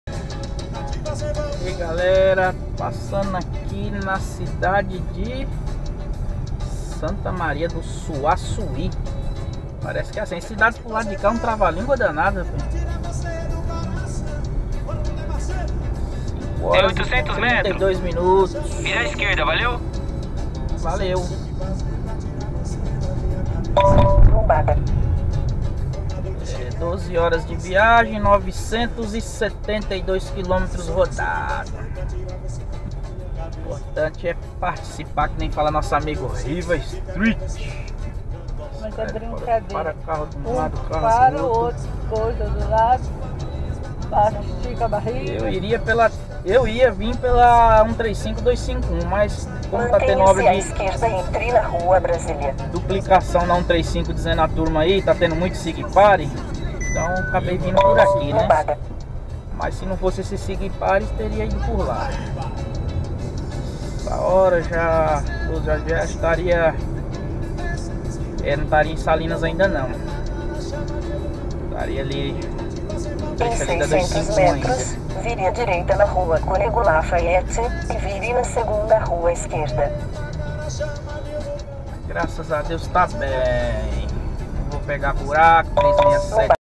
Viagem de Férias Agosto de 2021 de São Paulo a Virgem da Lapa MG Parte 9
Passando Pela Cidade de Santa Maria do Suaçuí